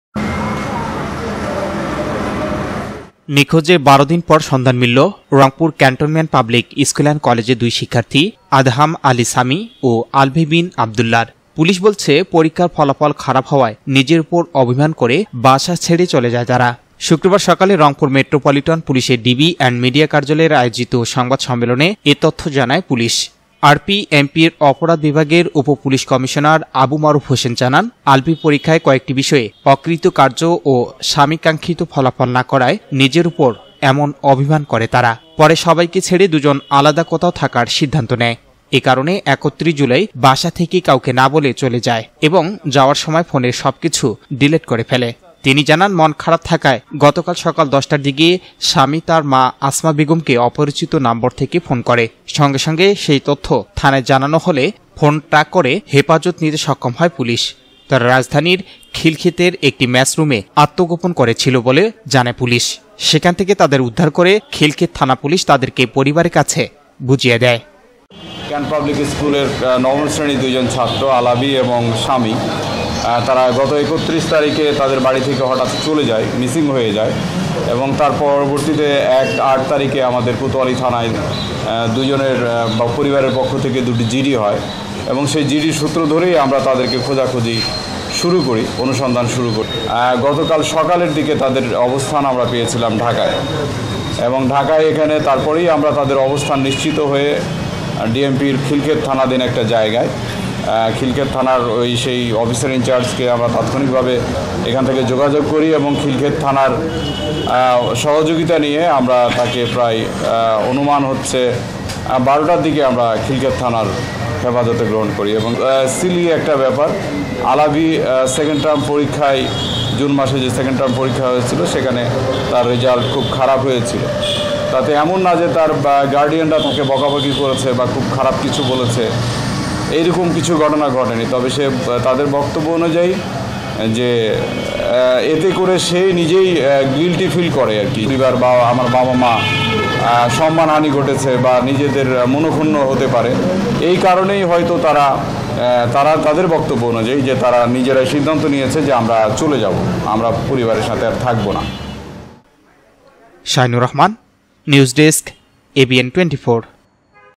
নিখোঁজের ১২ দিন পর সন্ধান মিলল রংপুর ক্যান্টনমেন্ট পাবলিক স্কুল এন্ড কলেজের দুই শিক্ষার্থীর
নিখোঁজের ১২ দিন পর সন্ধান মিলল রংপুর ক্যান্টনমেন্ট পাবলিক স্কুল এন্ড কলেজের দুই শিক্ষার্থী আদহাম আলী সামি ও আলভী বিন আব্দুল্লাহর। পুলিশ বলছে, পরীক্ষার ফলাফল খারাপ হওয়ায় নিজের উপর অভিমান করে বাসা ছেড়ে চলে যায় তারা।